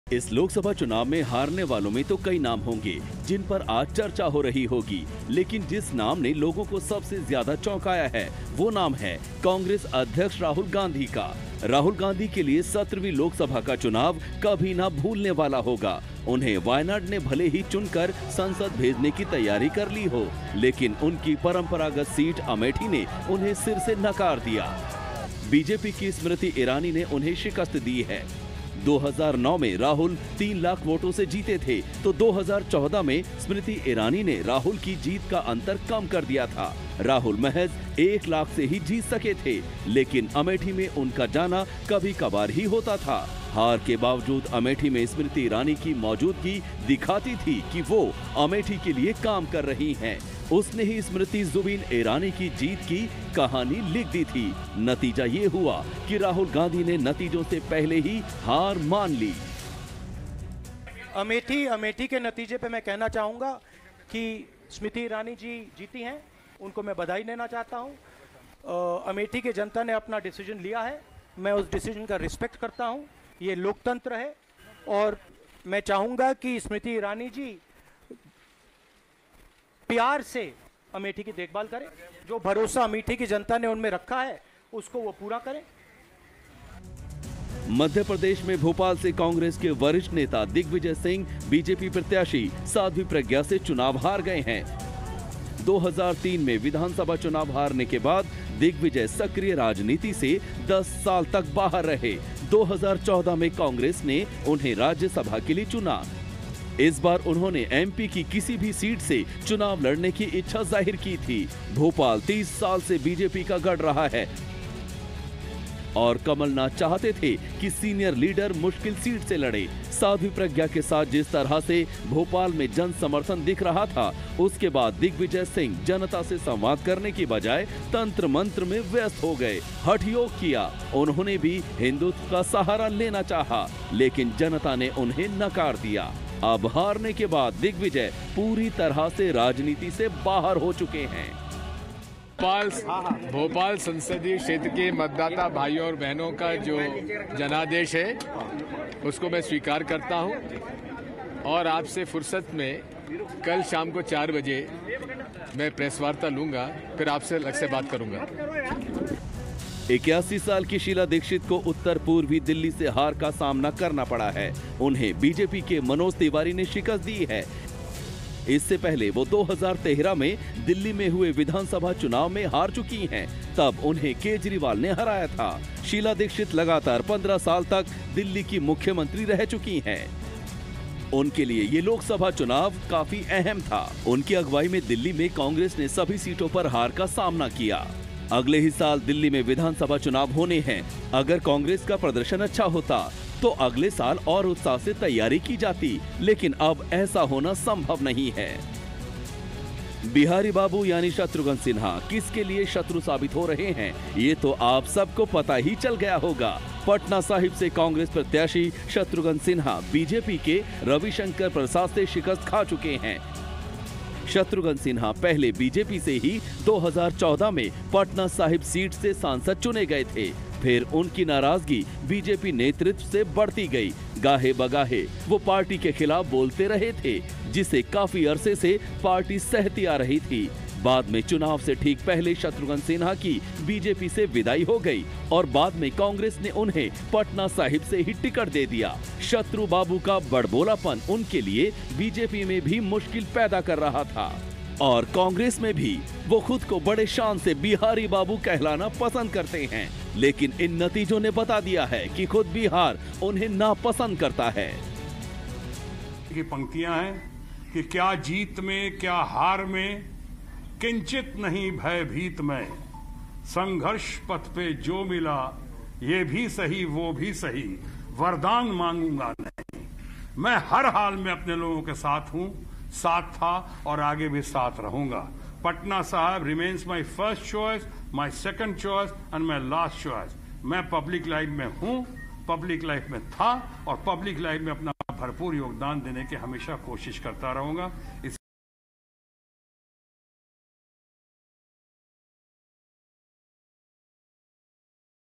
इन सीटों के चुनावी नतीजों ने खूब चौंकाया
इस लोकसभा चुनाव में हारने वालों के क्ई नाम है लेकिन कुछ सीटों के नतीजों ने खूब चौंकाया है.

About Channel